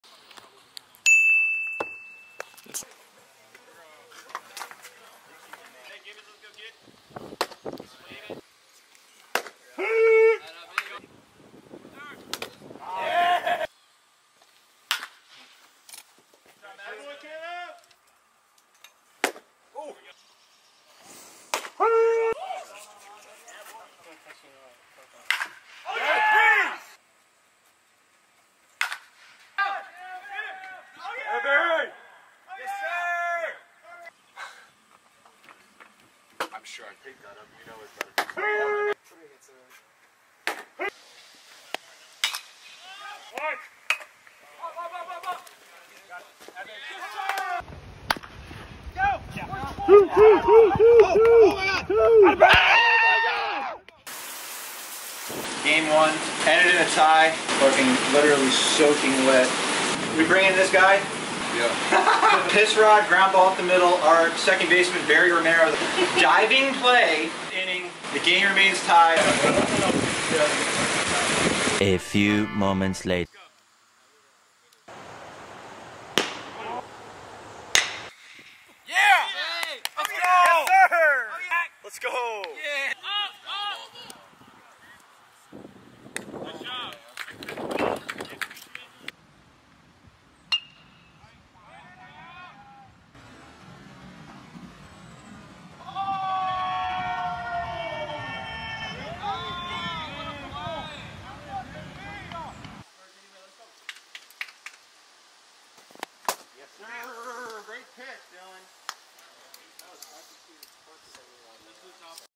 Mini Vlog Game 1 - Velo Farm World Series 2023
Game 1 Mini Vlog 📸
Game 1 of this year's world series was epic, entertaining, and competitive af. Both starters for City and the Sox shoved. Some big time plays late in the game kept the score even at 1-1. 😬
Going into the last inning we had a large storm system approaching. Tie ball game... 1-1, city boys are looking to walk it off... they have bases loaded with 2 outs. As the storm starts to open up, The Farm Sox get a ground ball up the middle, diving play by our 2nd basement... force out to end the inning. As we get off the field, the skys open up and it begins to literally hurricane on us. Luckily everyone was able to get to their cars as it got bad. Shout out to the baseball gods 🙏🏻⛈️
Play was suspended until the following day, where the Farm Sox took the lead, and won the game 2-1 in extras. Never a dull moment out here on the farm. Glad we were able to capture it all on camera.🤳
Last week for World series content, then back to the grind. Got some new and exciting stuff on the way.